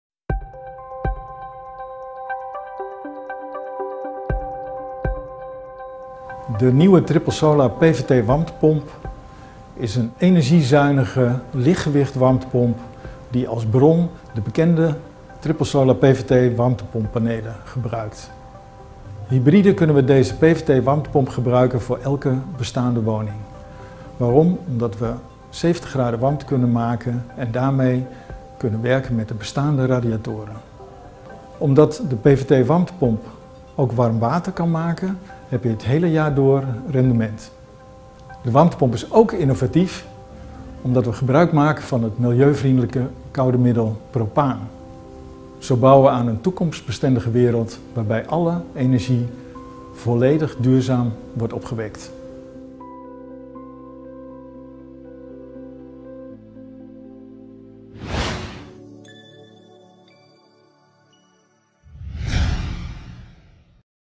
Hybride PVT warmtepomp van Triple Solar genomineerd VSK Award 2022 Cees Mager legt uit STEM OP ONS
Onze warmtepomp is genomineerd voor de VSK Award 2022

Deze nieuwe warmtepomp werkt samen met PVT-panelen op het dak, die zowel thermische energie als zonnestroom opwekken. Het lamelvormige thermisch deel van het paneel onttrekt energie uit de buitenlucht. De combinatie tussen zonlicht en buitenlucht als bron, en de compacte nieuw ontworpen warmtepomp op propaan (R290) maken het mogelijk om ook bij strenge vorst (tot -20 ˚C) aanvoertemperaturen tot 70 ˚C te realiseren. De warmtepomp leent zich voor nieuwbouw, waarbij het apparaat ook een koelfunctie heeft. Voor de renovatie is deze warmtepomp te combineren met de huidige cv-ketel en bestaand afgiftesysteem.

VSK JURYOORDEEL:

Een nieuwe compacte toepassing die breed en modulair toepasbaar is: van appartement in de nieuwbouw tot vrijstaande bestaande woning. Het feit dat met de techniek ook tussenstappen (hybride) gezet kunnen worden en de mogelijkheid bestaat tot koeling voor de nieuwbouw, ziet de jury als een pré. De techniek tackelt bovendien een belangrijke consequentie van de klassieke lucht/water-opstelling: de buitenunit. Het PVT-paneel als bron van duurzame techniek ziet de jury als een blijvertje in de energietransitie. Prima doorontwikkeling dus.